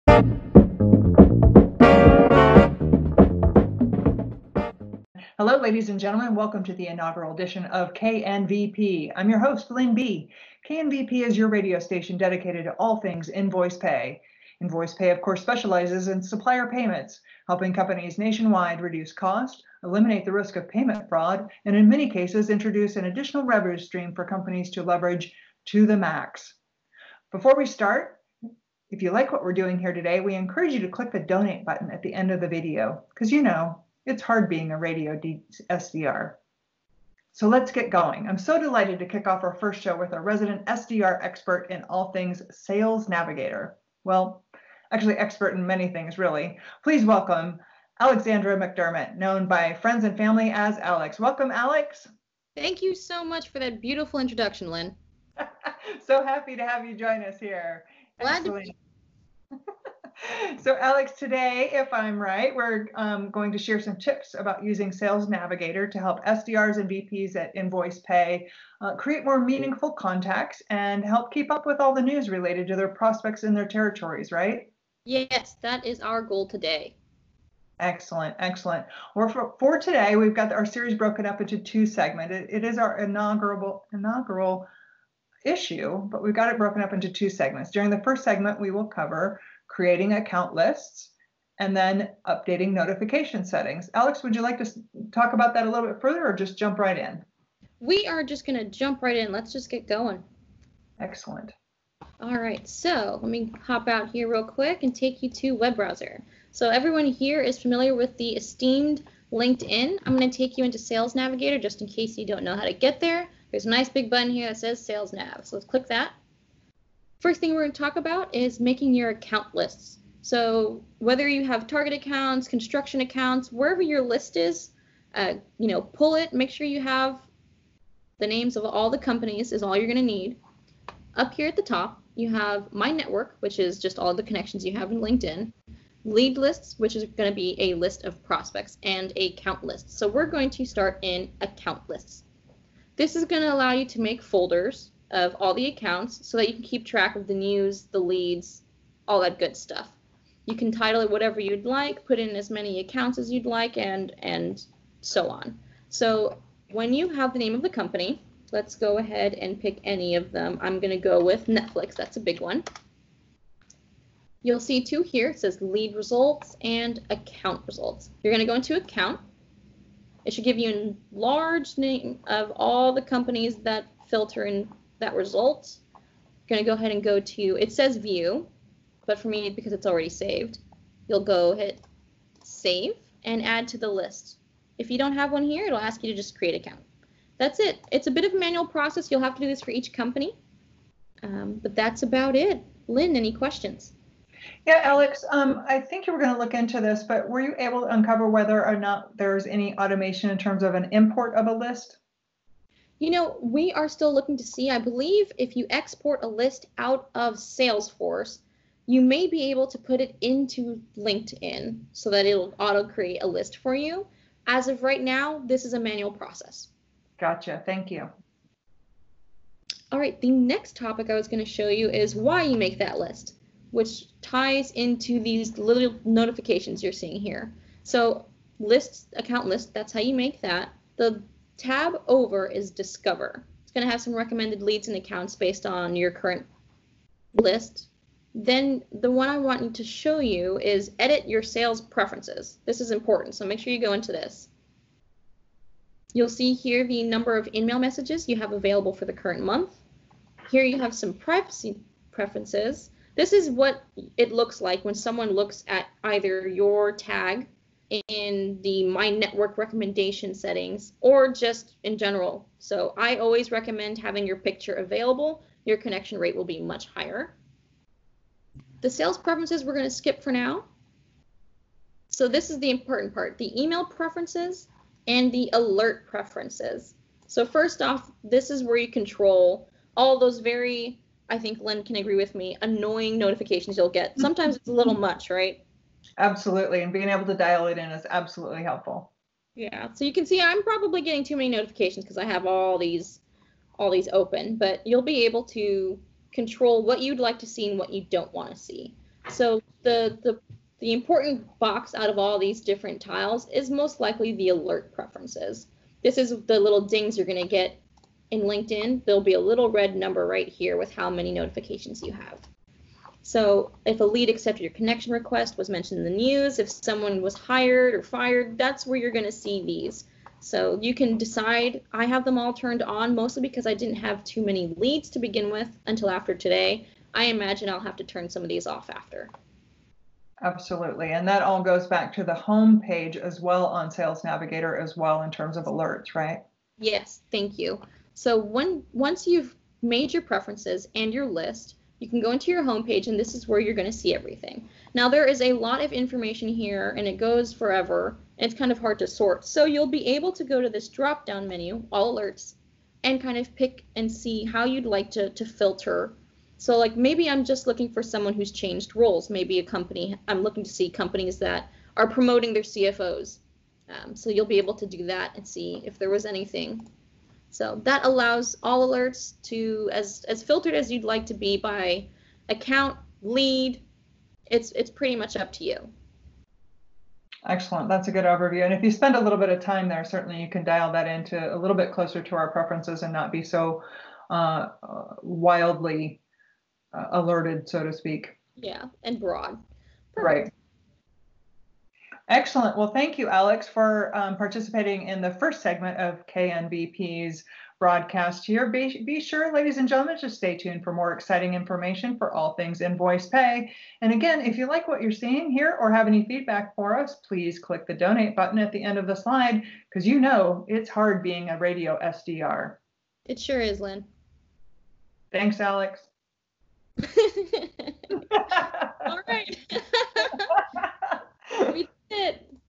KNVP | Episode 1 Sales Navigator Part 1
Lynn and Alex walk you through how to save accounts in sales navigator and how to edit your notification preferences.